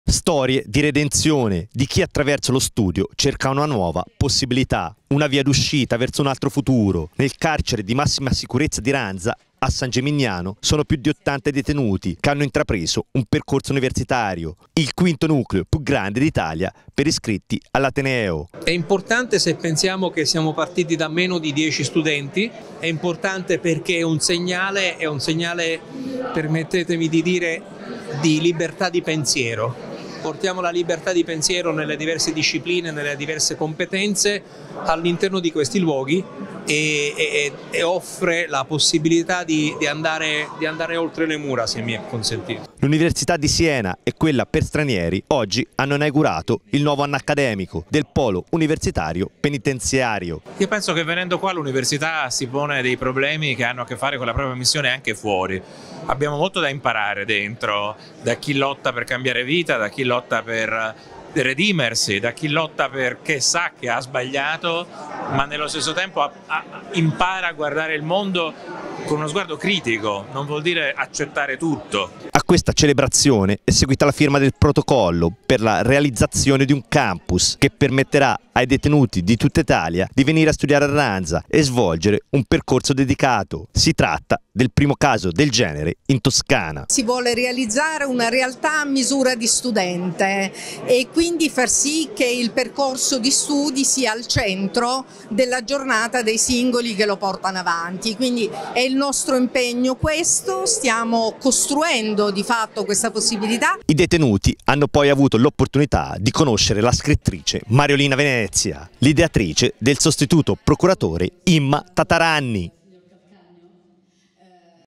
Aperto il nuovo accademico nel penitenziario di Ranza: presentato anche il campus universitario
Inaugurato nel carcere di Ranza, a San Gimignano, il nuovo anno accademico del Polo universitario penitenziario, che coinvolge sia l'Università di Siena che l'Università per stranieri di Siena. Realtà che conta più di 80 studenti tra i detenuti ed è quinta in Italia per iscritti. L'occasione anche per presentare il campus universitario, che prevede un percorso dedicato per i detenuti e rappresenta il primo caso del genere in Toscana